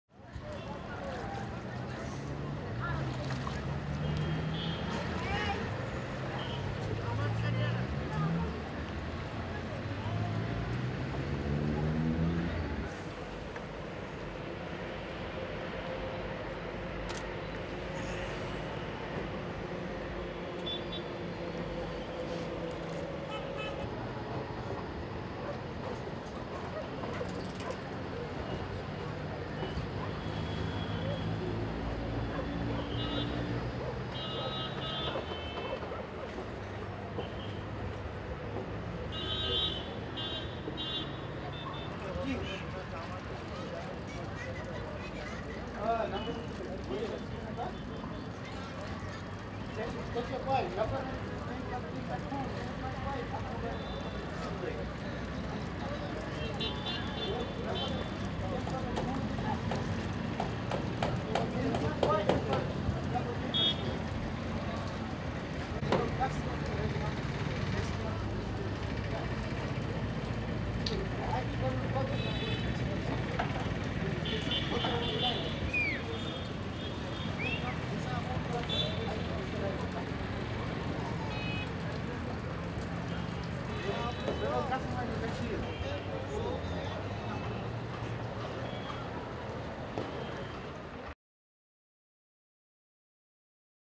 Evening Shikara rides by tourists on the Dal Lake
Houseboats are a great place to see the life on the Dal Lake. Evening Shikara rides by tourists, traders selling craft items and artificial jewellery to their prospective customers riding shikaras, boatmen manoeuvring their boats back to their homes, are some of the insightful sights one gets to see from a houseboat.